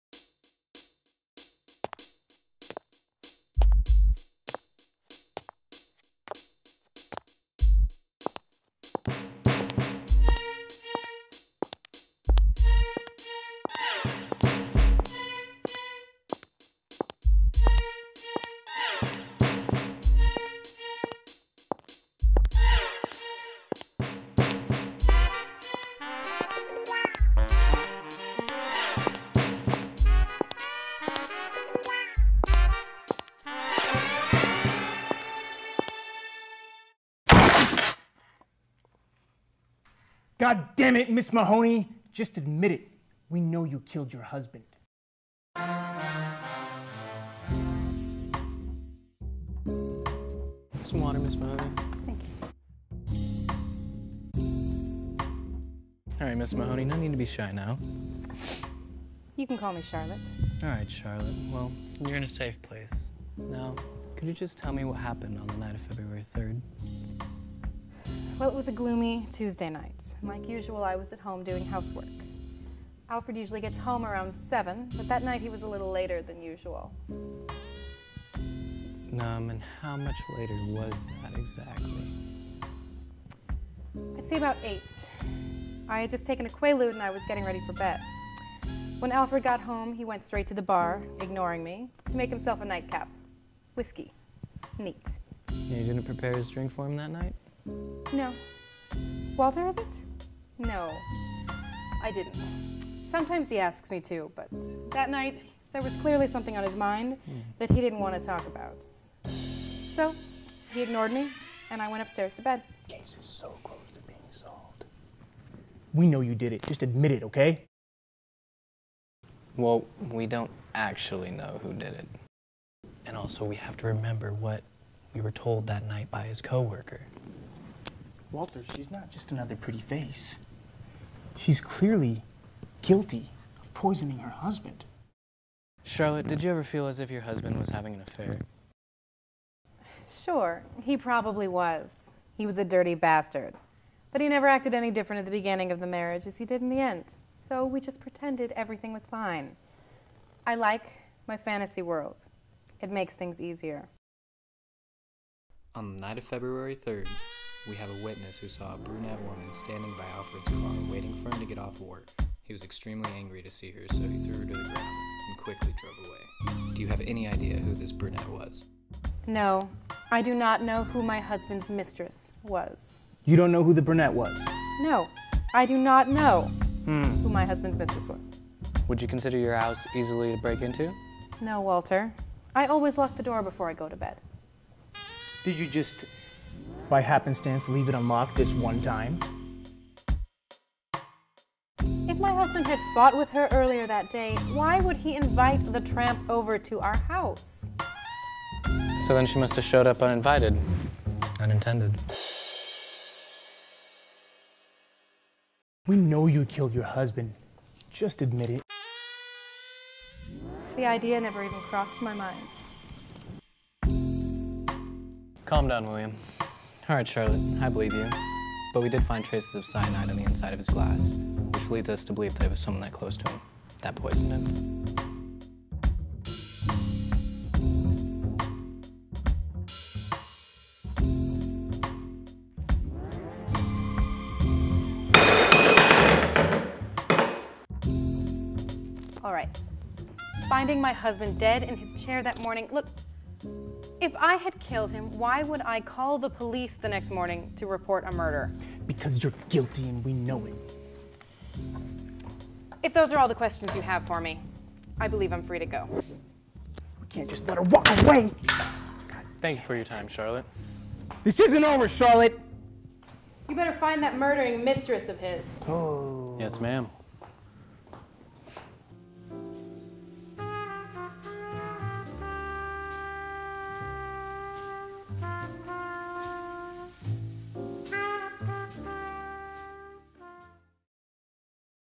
Whiskey,Neat
Video Production 215 Final

Inspired by Bill Wilder film 'Double Indemnity'

Shout out to my brother Dominic for creating the music that sets that film noir mood ~